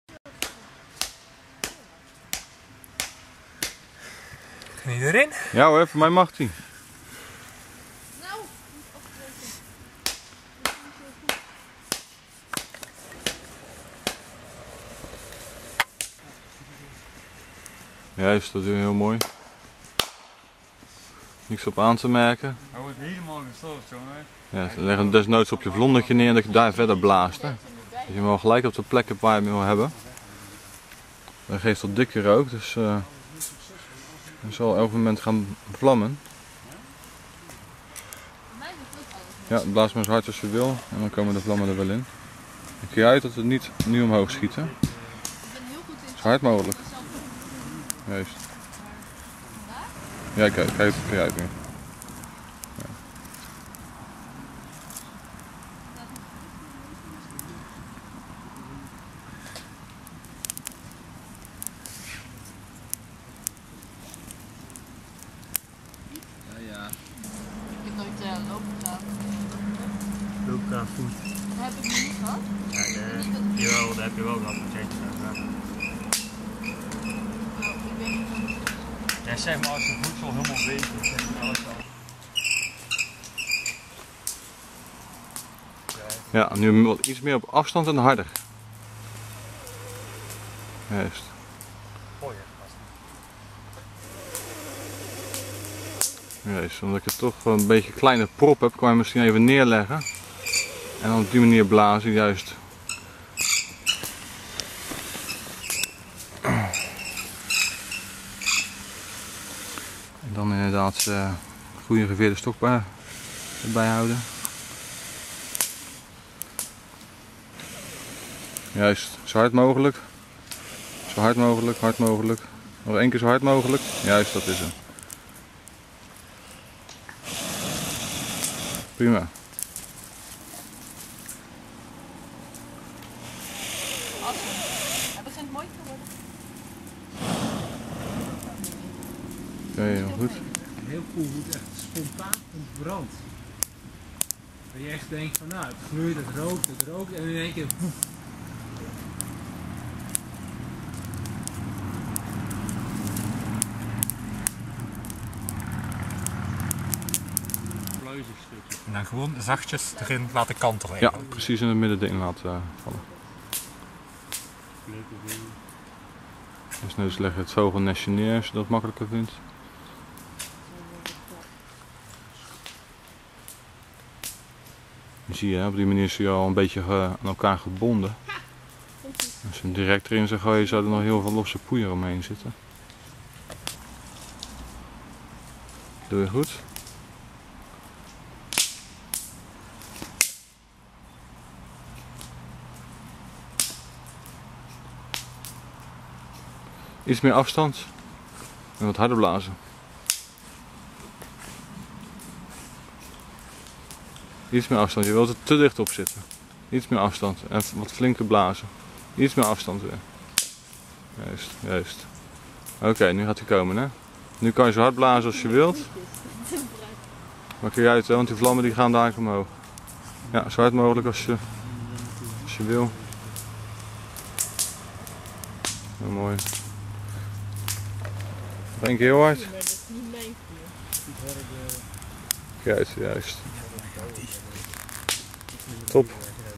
Deelnemers zijn succesvol met de vuurboog | Wildernisschool Outback
Vandaag zijn we gestart met de bushcraftweek in België en aan het einde van de dag kwamen twee technieken van vuur maken aan bod waaronder vuur maken met de vuurboog. Drie van de zes deelnemers waren hiermee zelfstandig succesvol en zelfs twee van hun breidde hun vuur uit met behulp van geveerde stokken.